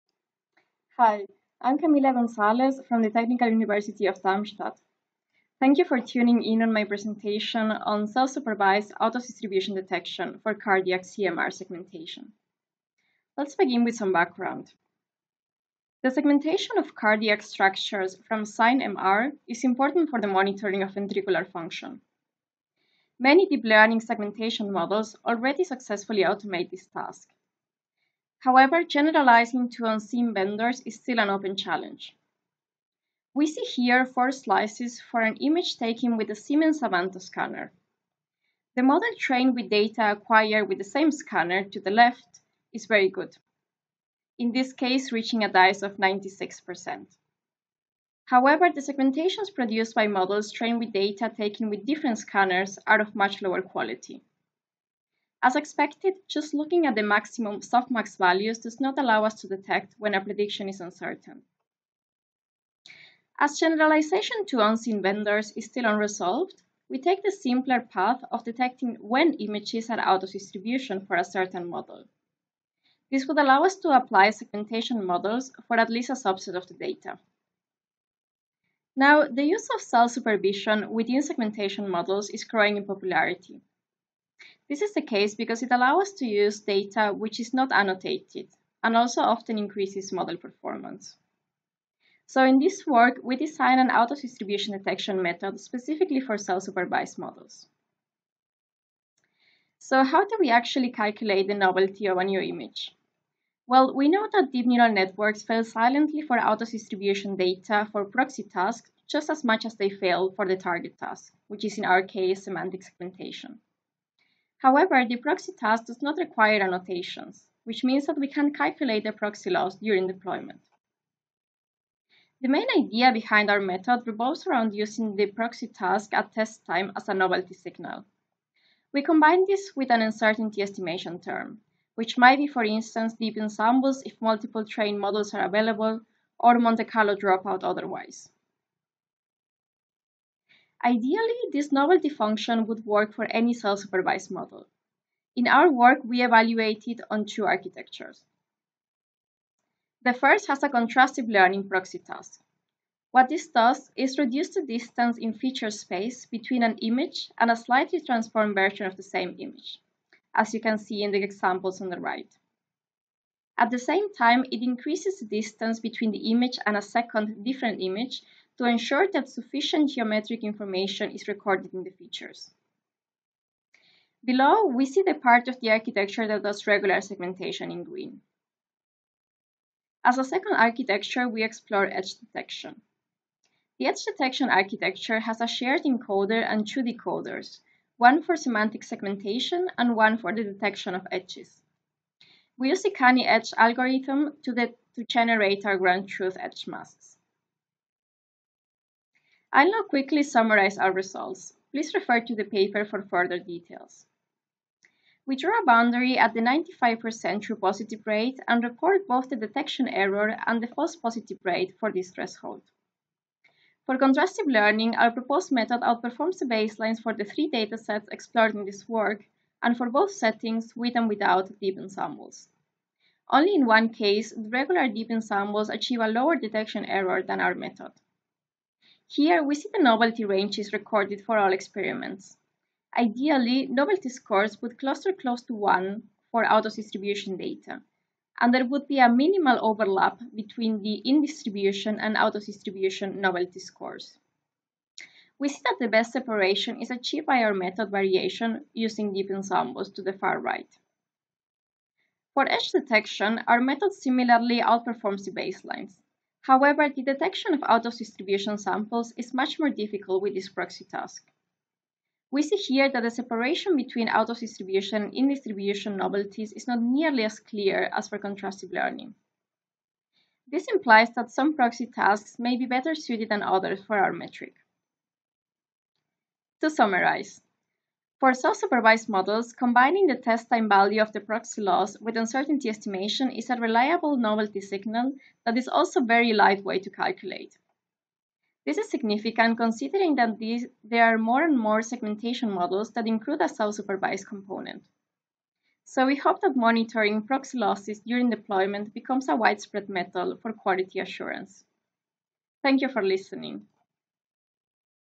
MIDL 2021, J9, Gonzalez et. al., Full Paper
J9 - Self-supervised Out-of-distribution Detection for Cardiac CMR Segmentation

Camila Gonzalez, Anirban Mukhopadhyay

Abstract: The segmentation of cardiac structures in Cine Magnetic Resonance imaging (CMR) plays an important role in monitoring ventricular function, and many deep learning solutions have been introduced that successfully automate this task. Yet due to variabilities in the CMR acquisition process, images from different centers or acquisition protocols differ considerably. This causes deep learning models to fail silently. It is therefore crucial to identify out-of-distribution (OOD) samples for which the trained model is unsuitable. For models with a self-supervised proxy task, we propose a simple method to identify OOD samples that does not require adapting the model architecture or access to a separate OOD dataset during training. As the performance of self-supervised tasks can be assessed without ground truth information, it indicates during test time when a sample differs from the training distribution. The proposed method combines a voxel-wise uncertainty estimate with the self-supervision information. Our approach is validated across three CMR datasets and two different proxy tasks. We find that it is more effective at detecting OOD samples than state-of-the-art post-hoc OOD detection and uncertainty estimation approaches.